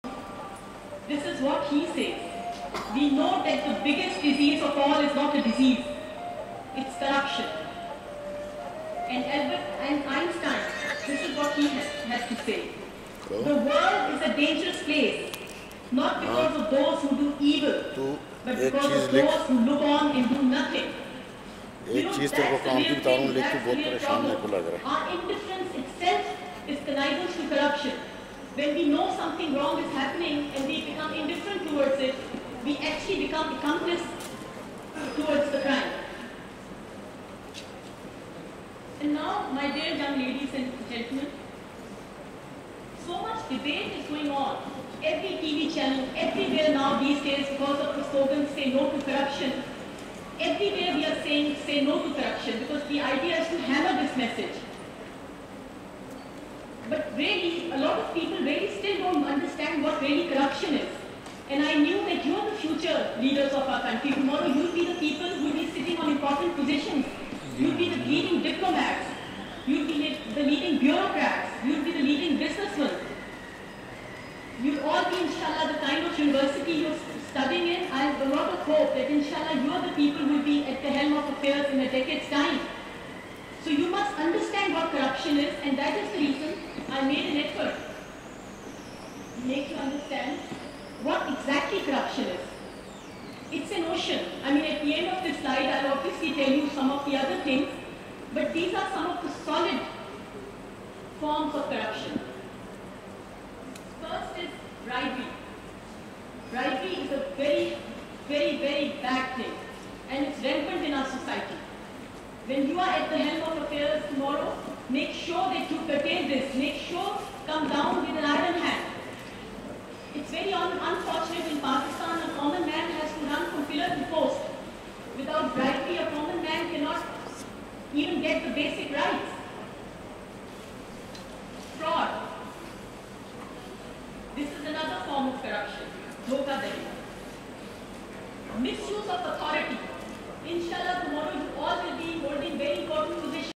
Aliya Rashid delivering a Lecture at Quaid-i-Azam University on Say NO to Corruption
Say NO to Corruption